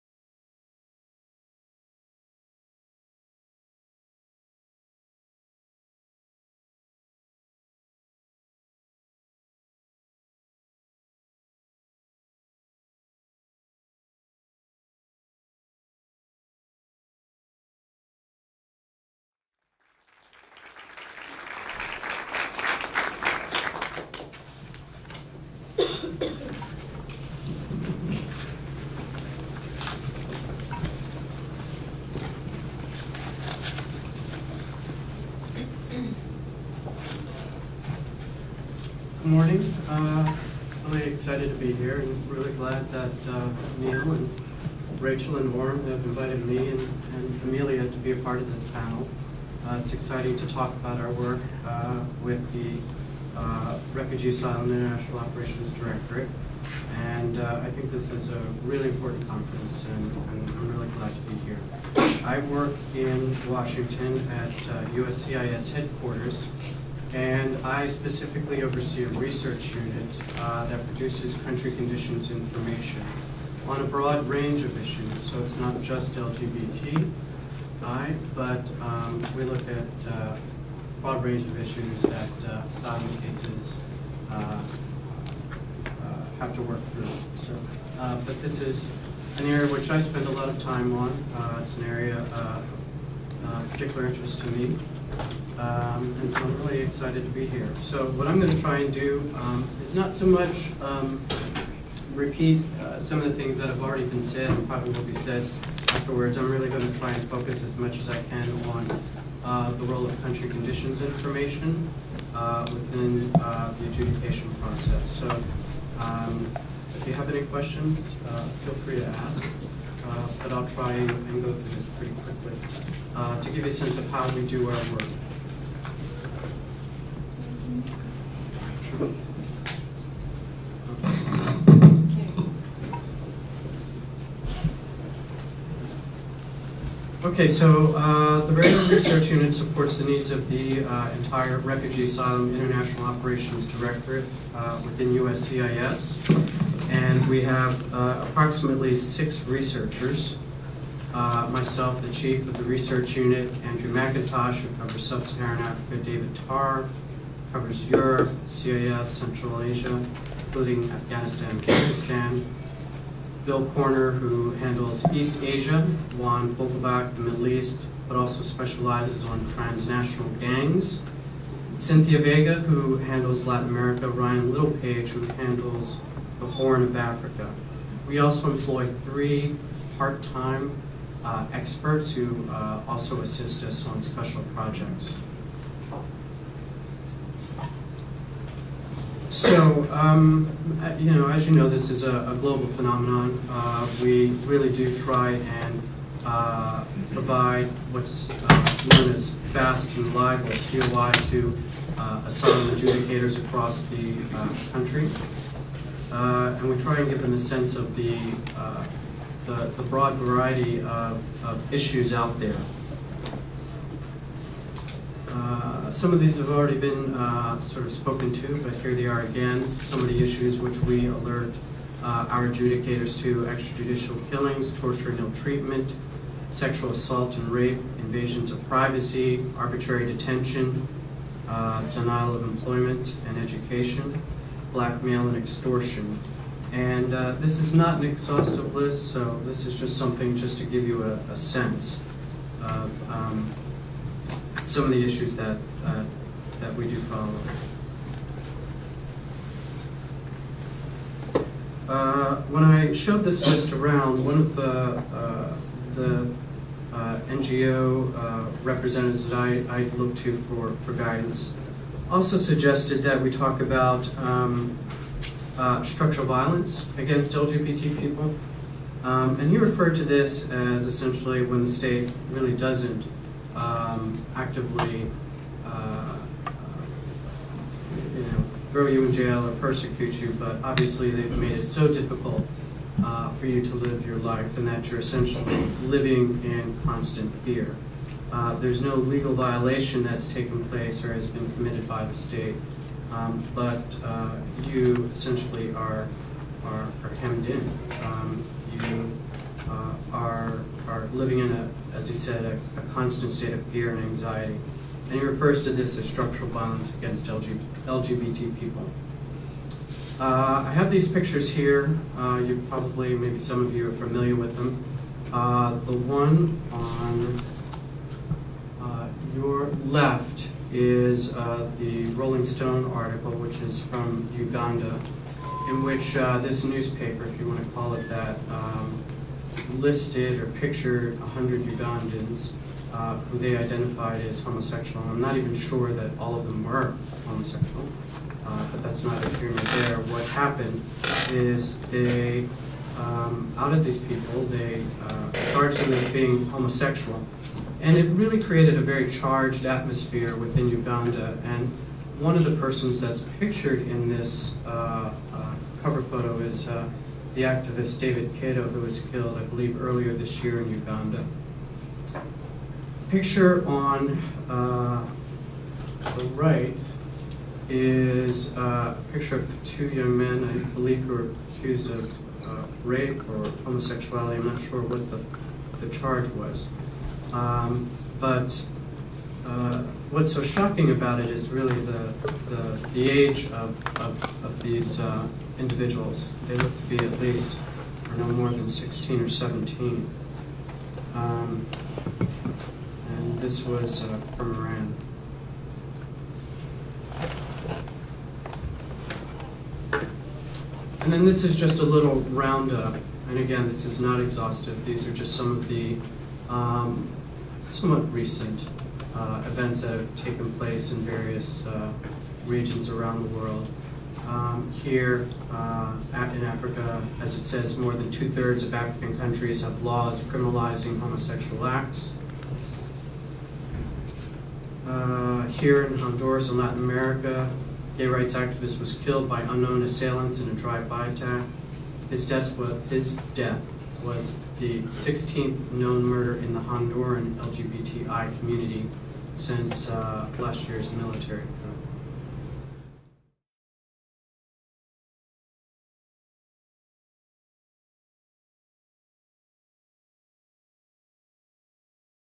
Adjudication of LGBTI Asylum Claims (Part 3)
LeRoy Potts -- Chief of Research for the San Francisco Asylum Office within the Refugee, Asylum and International Operations Directorate of the U.S. Department of Homeland Security -- provides unique governmental views on the interpretation of LGBTI claims for asylum. This lecture was delivered June 24, 2011, in San Francisco, CA.